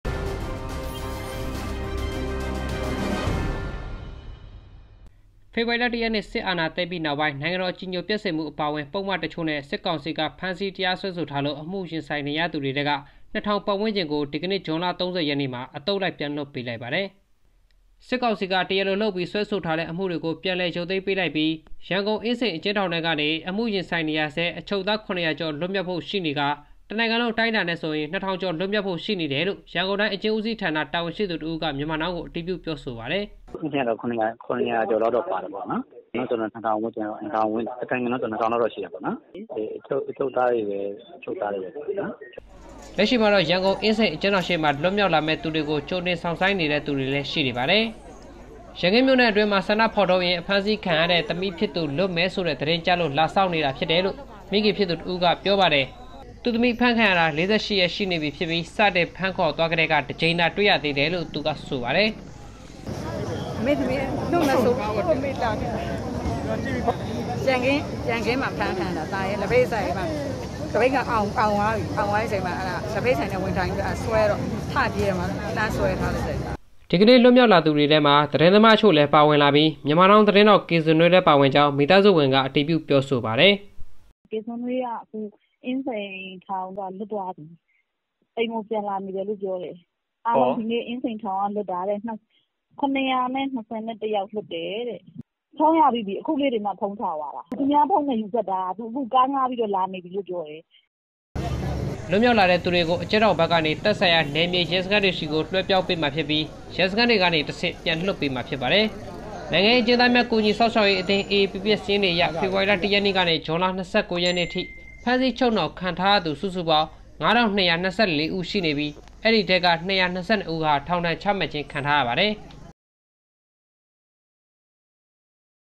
စစ်ကောင်စီက တရားစွဲဆိုထားသူထဲမှ နှစ်ထောင်ခန့်ကို ပြန်လွှတ်ပေး
ဖေဖေါ်ဝါရီ ၁ ရက်နေ့ စစ်အာဏာသိမ်းပြီးနောက်ပိုင်း နိုင်ငံတော်အကြည်ညိုပျက်စေမှုအပါအဝင် ပုဒ်မအချို့ဖြင့် စစ်ကောင်စီက ဖမ်းဆီးတရားစွဲဆိုထားပြီး အမှုရင်ဆိုင်နေရသူများထဲမှ ၂,၀၀၀ ခန့်ကို ယနေ့ ဇွန်လ ၃၀ ရက်နေ့တွင် အသုတ်လိုက်ပြန်လွတ်ပေးလိုက်ပါသည်။

ယနေ့ လွှတ်မြောက်လာသူများထဲတွင် သတင်းသမားအချို့လည်းပါဝင်လာပြီး Myanmar now သတင်းထောက် ကေဇွန်နွေးလည်းပါဝင်ကြောင်း မိသားစုဝင်က အတည်ပြုပြောဆိုပါသည်။